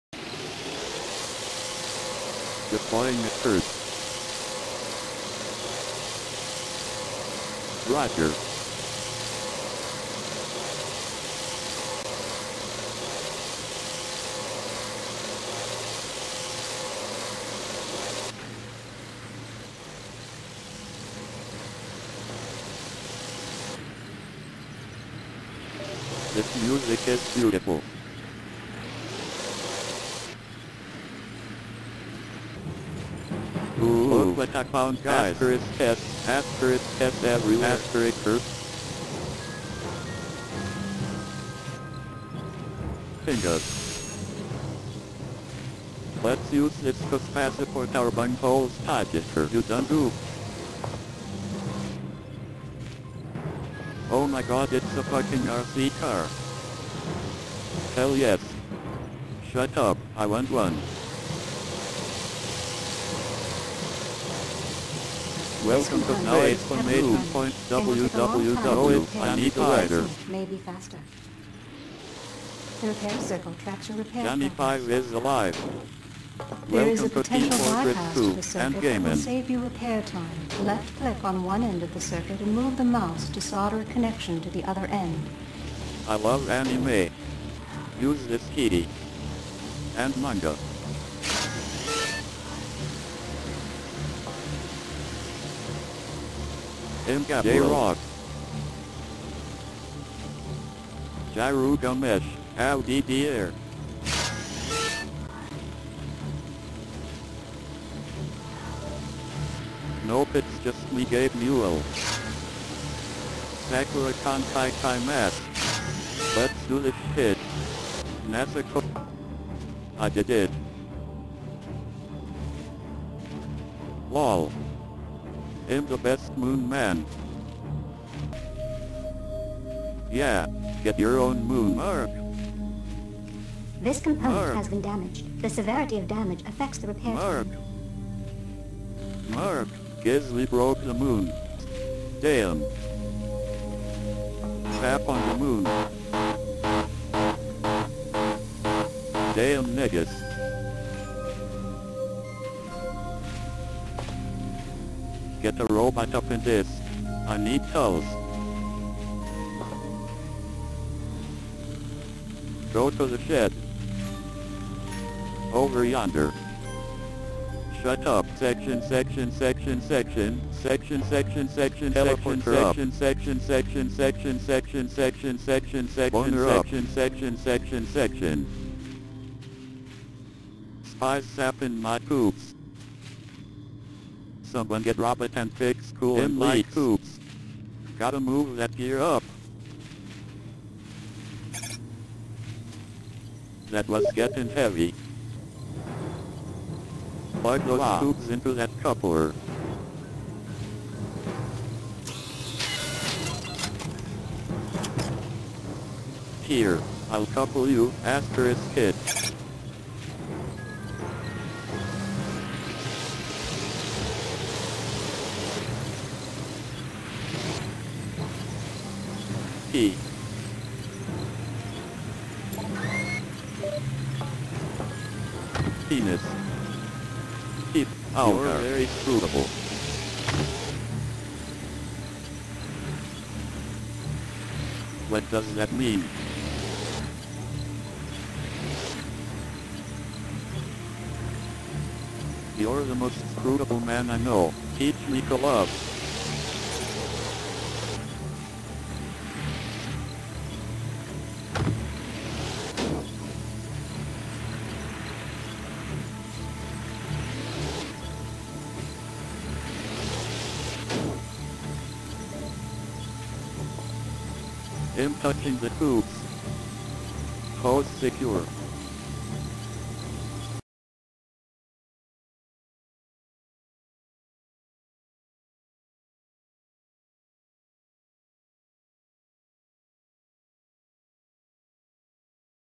We're morons and we're on the moon
trying to fix a  moon base while you're surrounded by literal retards is impossible, literally. moonbase more like piece of shit that is broken